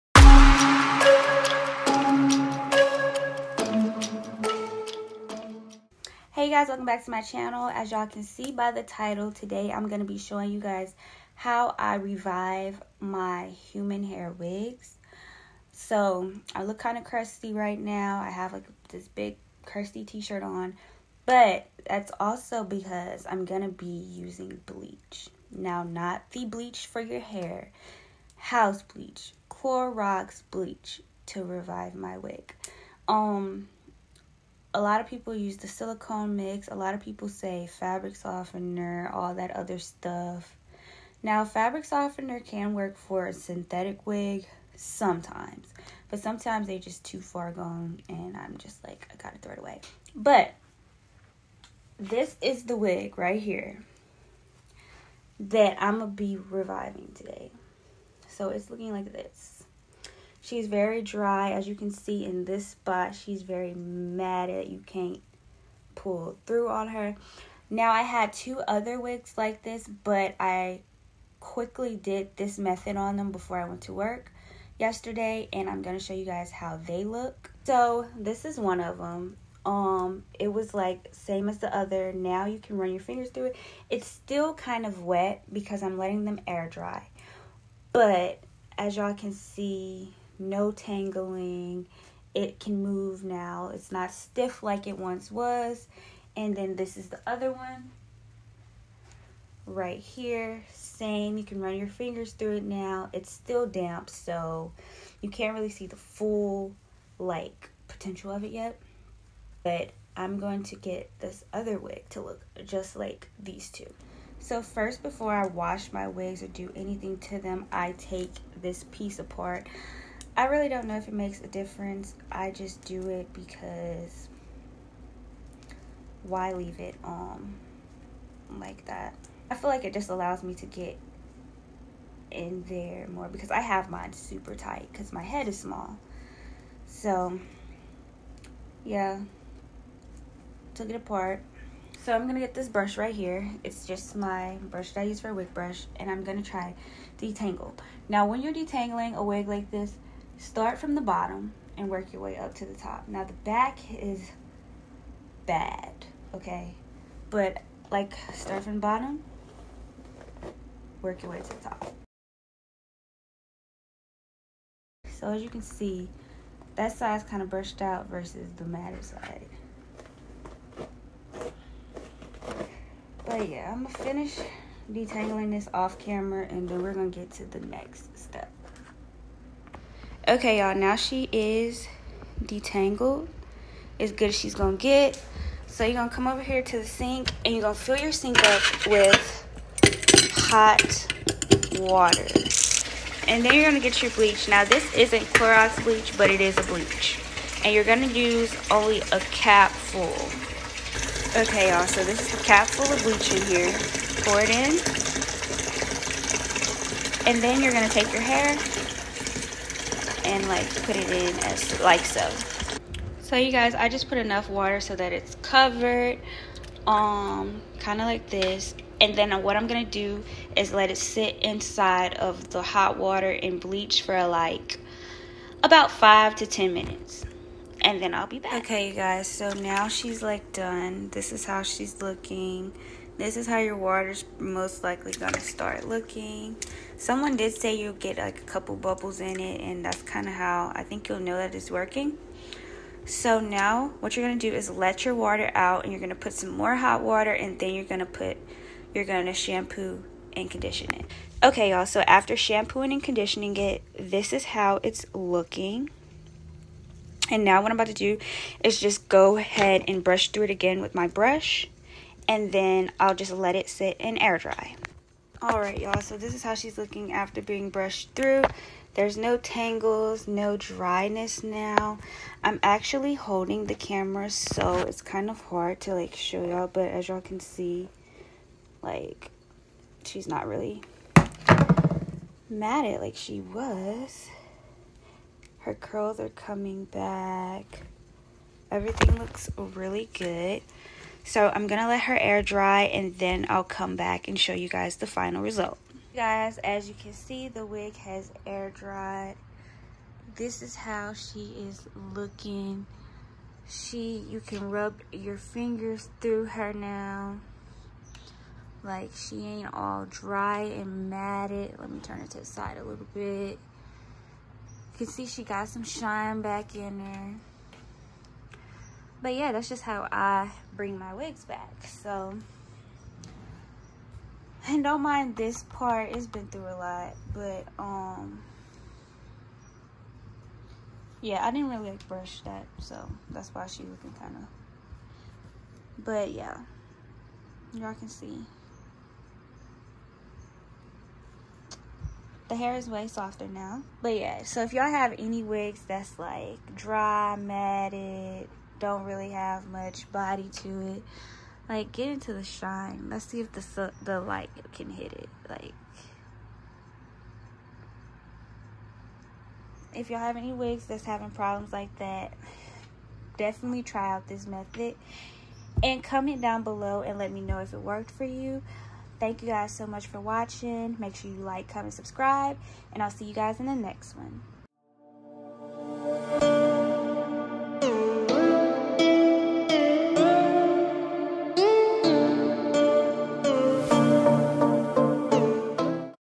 MAKE YOUR OLD WIGS LOOK NEW | REVIVE & RESTORE OLD HUMAN HAIR WIGS| NIA VONE
♡PRODUCTS USED♡

 COMING SOON

♡FAQ♡
name: nia
camera: iphone 11
editing software: imovie

♡KEY WORDS♡